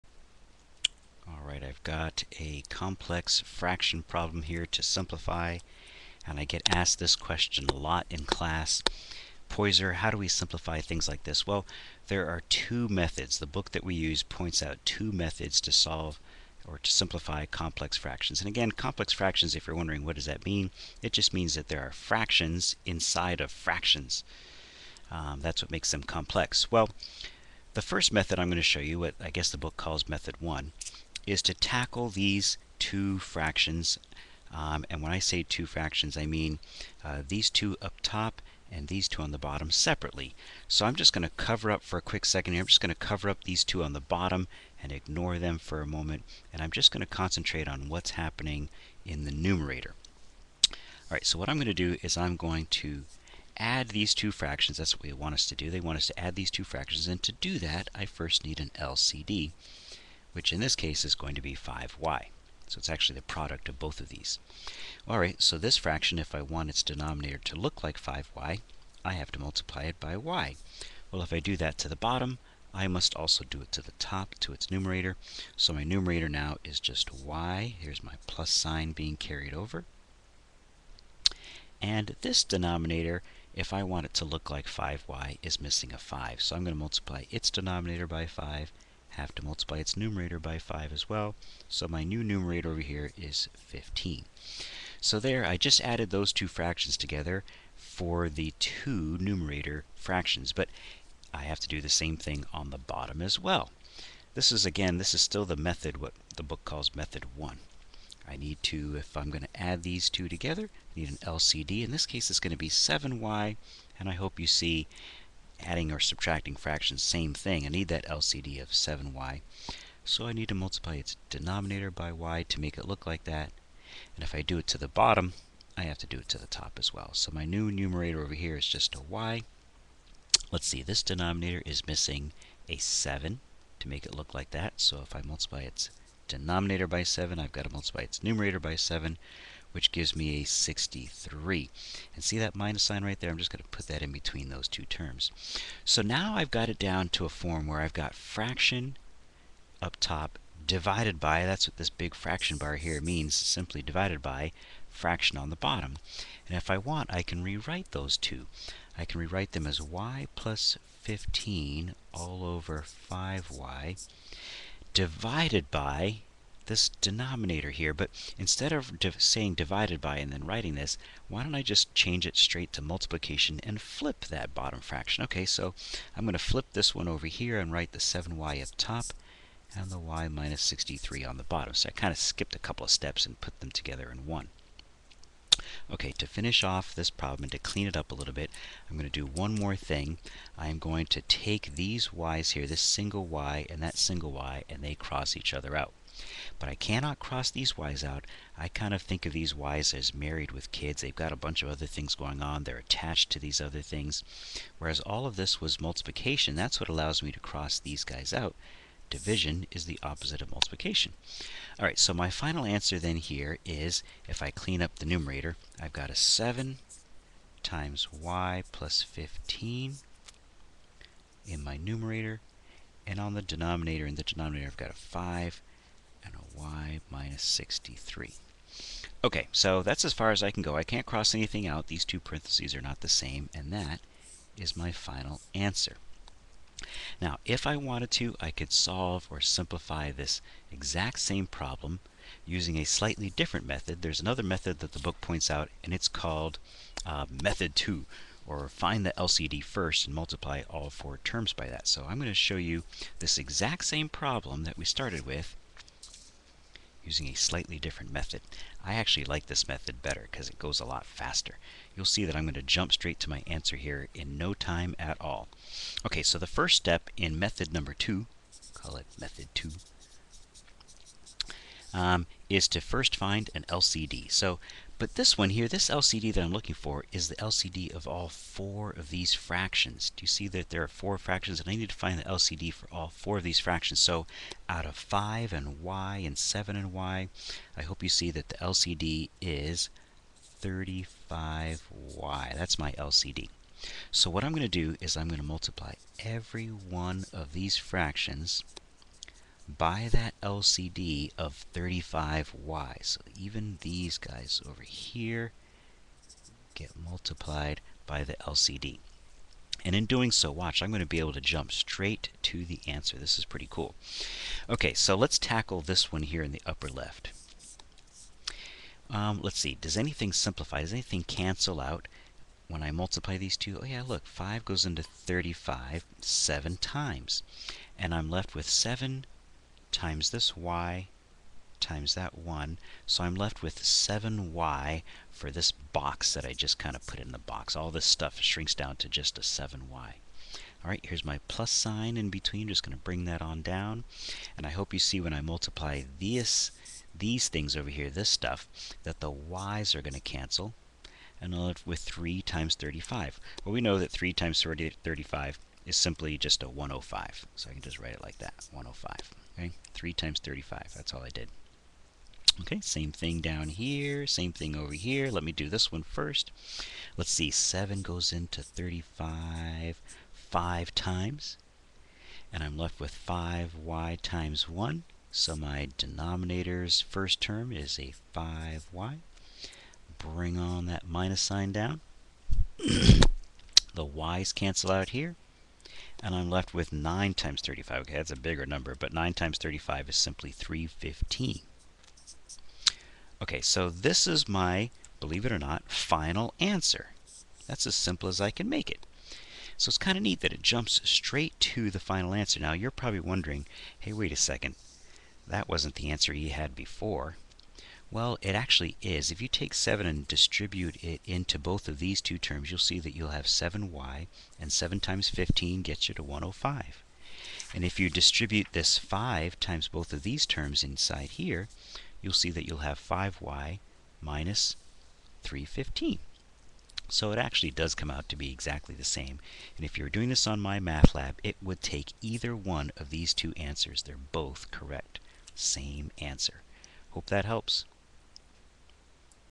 Simplifying Complex Fractions made up of 4 "small" fractions
Demonstrates two methods for simplifying complex fractions made up of four "small" fractions.

Method 1:  combine the two fractions in the numerator, then combine the two fractions in the denominator. Finally, flip the bottom fraction and multiply.

Method 2:  find the LCD of all four fractions, then multiply each of the four fractions by the LCD.